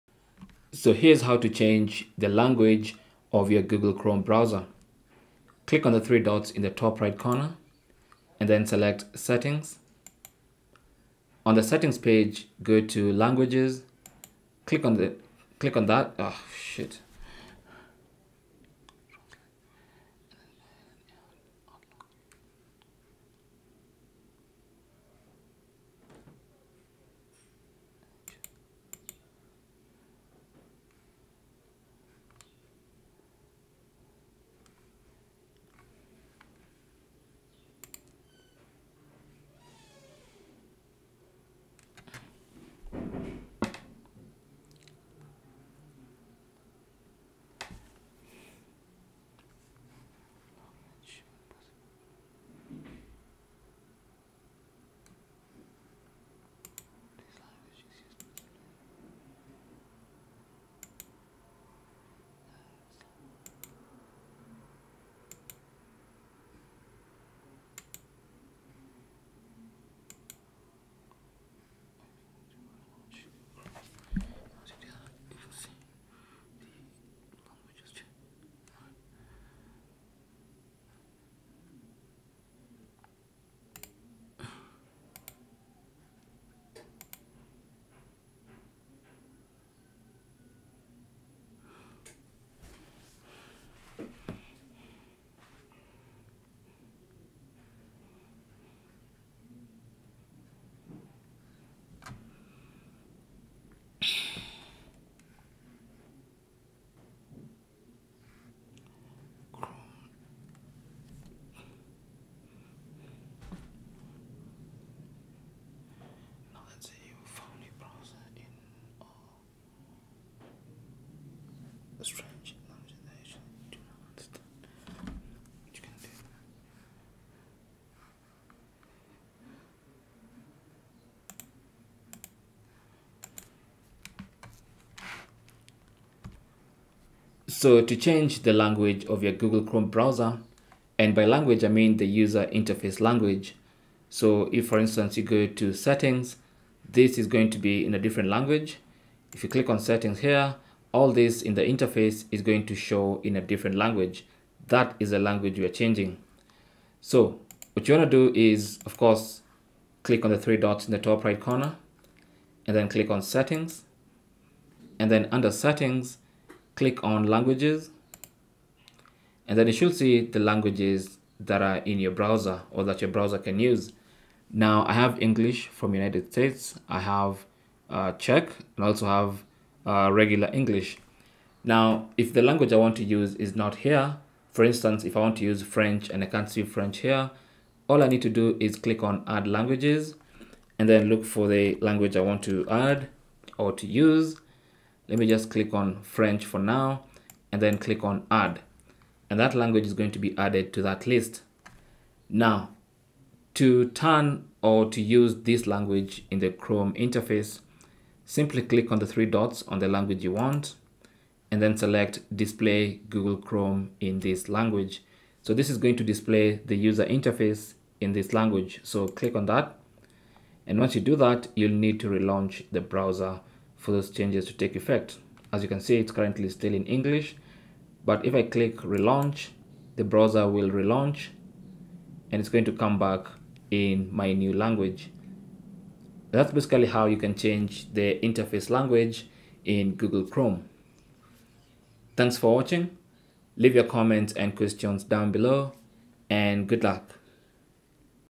How To Change Language In Google Chrome (Change Chrome UI Language)
Find out how you can change the interface language in Google Chrome. Use any available language of your choice. Simply make sure that the language is actually on Google's database for this to work.

Cell Phone Tripod Adapter